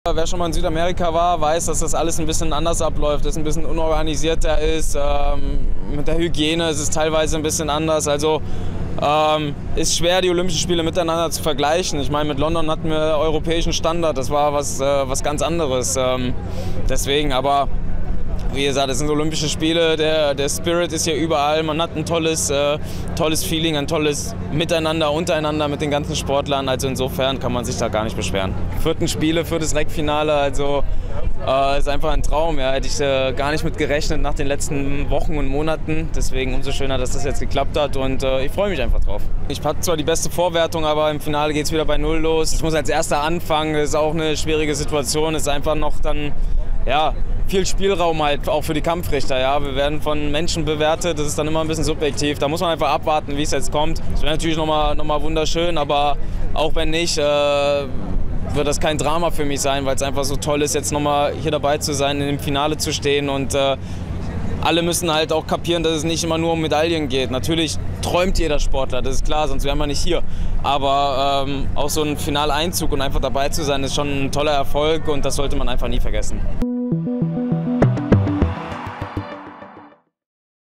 Hambüchen über Brasilien: "Alles unorganisierter"
Kunstturner Fabian Hambüchen kämpft am Dienstag im Reck-Finale erneut um eine olympische Medaille. Die Rahmenbedingungen in Rio de Janeiro sind allerdings nicht die einfachsten.